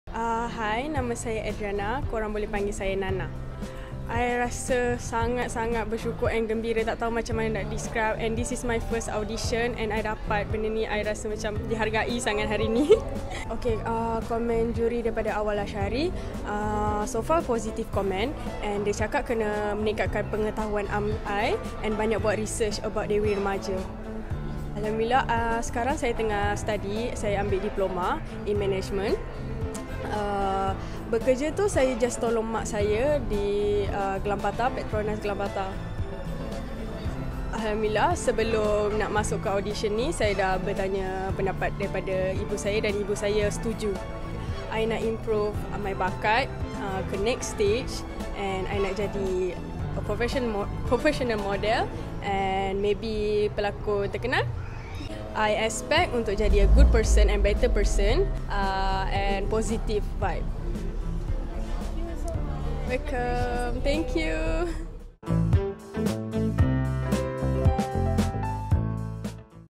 Peserta Pertama Ujibakat Dewi Remaja Mendapat Kad ‘Tahniah’
Tahniah kepada Adriana kerana berjaya mendapatkan kad ‘Tahniah’ di Ujibakat Dewi Remaja 2022 yang bertempat di Johor Bahru.